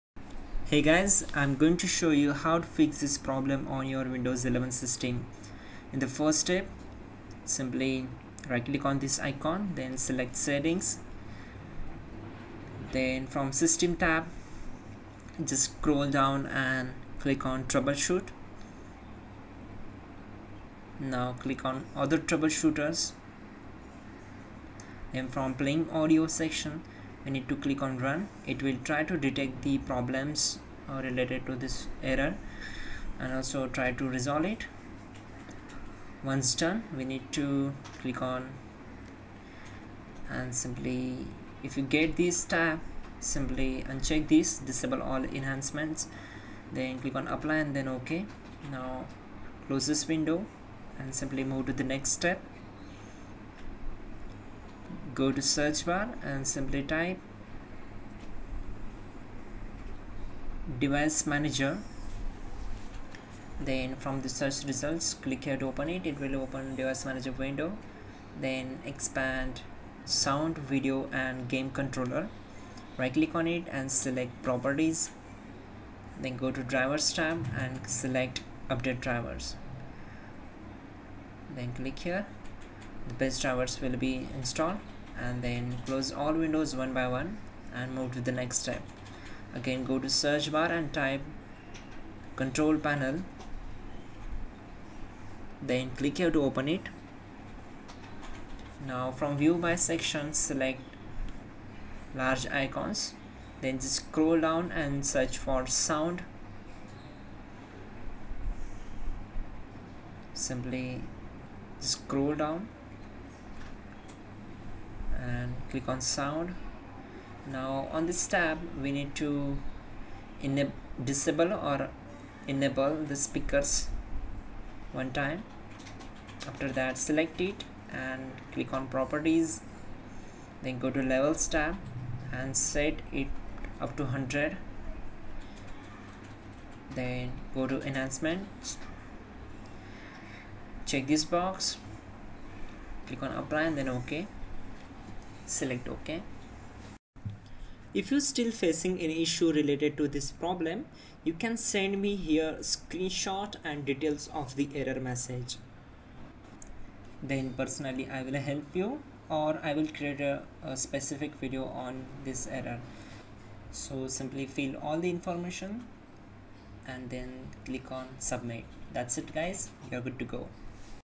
How to Fix Audio Problem in HP Laptop Windows 11
Lets Learn How to Fix Audio Problem in HP Laptop Windows 11
There are many locations where sound problems can happen. It might be your headphones or speakers, your operating system or audio software, your sound device or its driver.

Fortunately, many Windows sound issues can be fixed by simply turning up the volume or selecting the appropriate audio device from the programme you're using.

It is fairly simple to determine if you have no sound in Windows, but it is also possible that the sound only occasionally functions, with specific devices, or within a specific programme. How to Fix Audio Problem in HP Laptop Windows 11